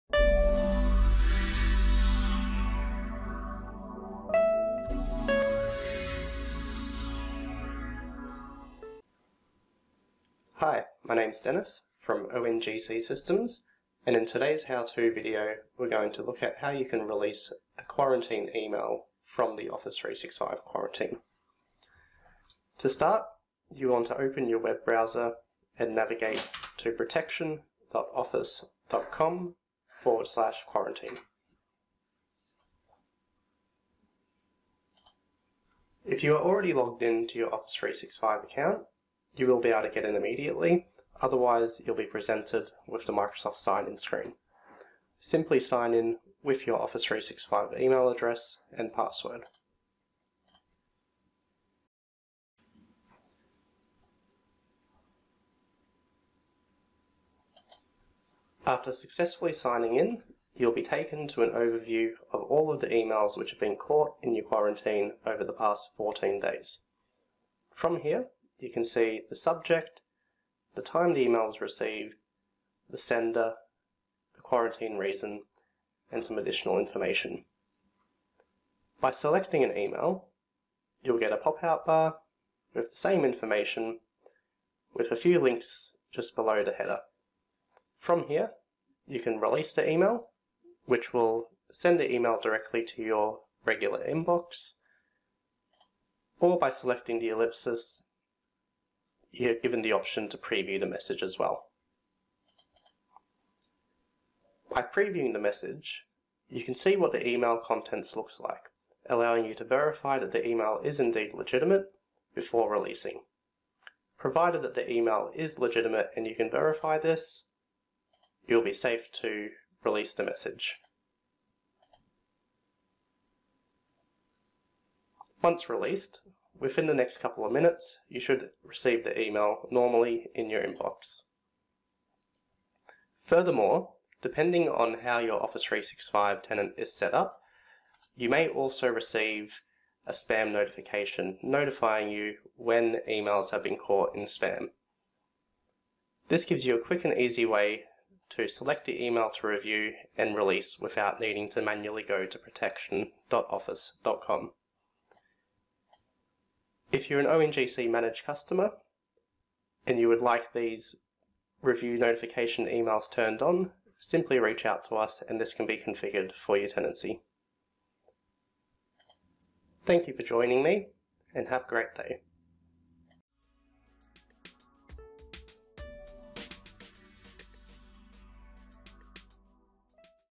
How To Release An Email From Quarantine
If you need to learn how to release an email from quarantine, we have you covered. Check out this video on releasing your emails from quarantine. 📧